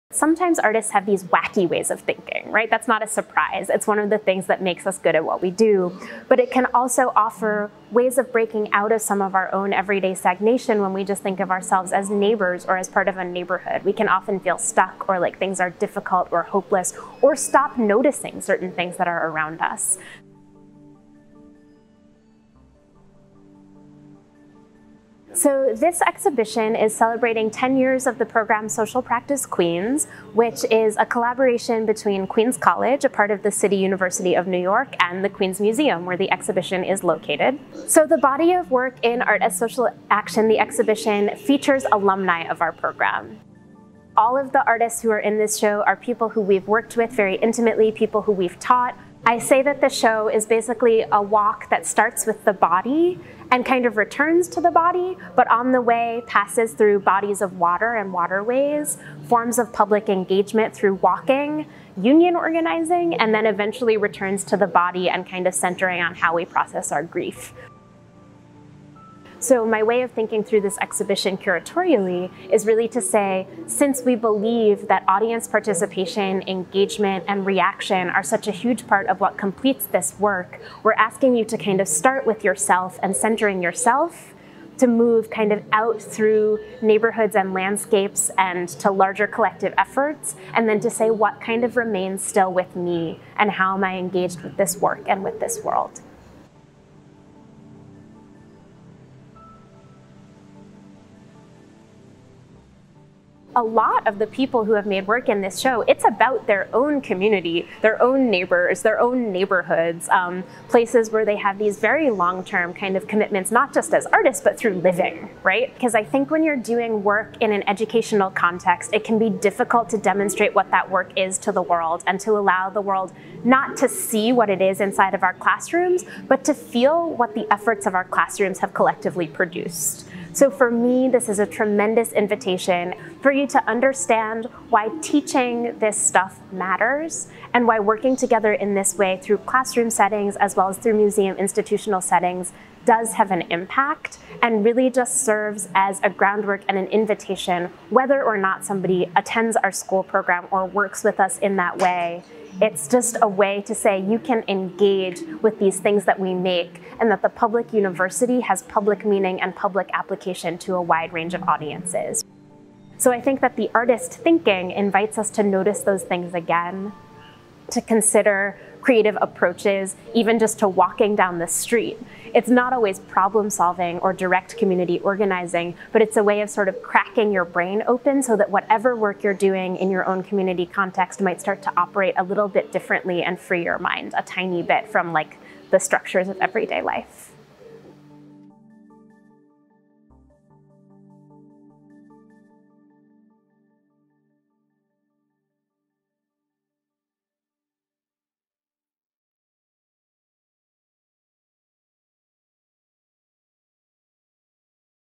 "Art As Social Action: 10 Years of Social Practice Queens"
Social Practice Queens (SPQ) is a unique degree program and partnership between Queens College CUNY and the Queens Museum, focused on the intersection of art and social justice. "Art As Social Action: 10 Years of Social Practice Queens" honors this decade-long pedagogical experiment by featuring interdisciplinary projects by nine local and international SPQ alumni: Alix Camacho-Vargas, Floor Grootenhuis (with Joel Murphy), Cody Herrmann, Jeff Kasper, Naomi Kuo, Julian Louis Phillips, Erin Turner, Pedro Felipe Vintimilla Burneo, and the Workers Art Coalition.

"Art As Social Action: 10 Years of Social Practice Queens" in on view at the Queens Museum From March 24, 2021 to July 25, 2021.